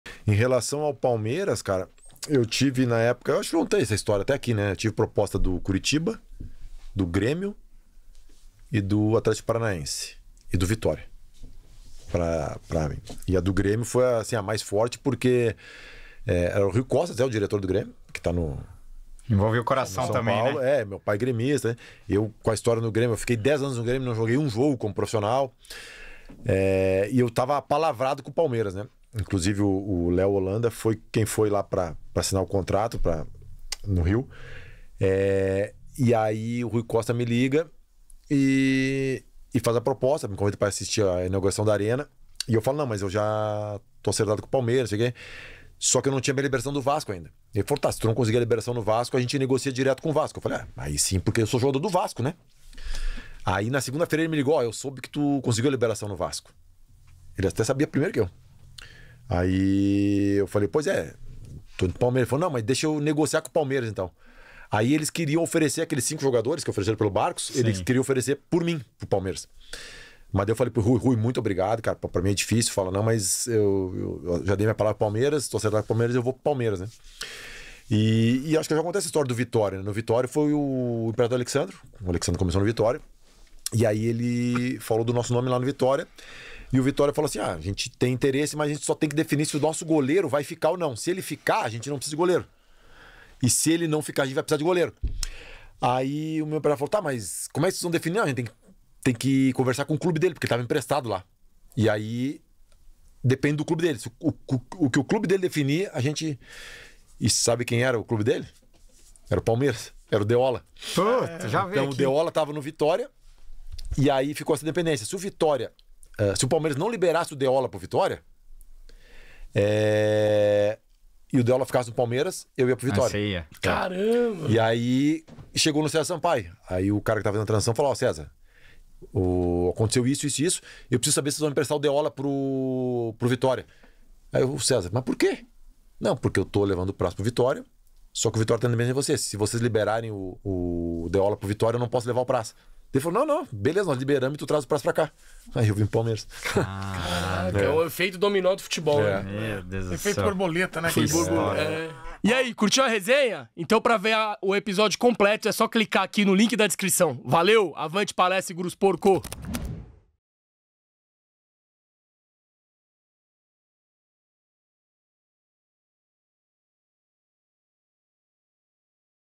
VC SABIA? PRASS SÓ VEIO PRO PALMEIRAS POR CAUSA DO DEOLA...
Assista ao episódio completo com Fernando Prass e Edu de Meneses neste link: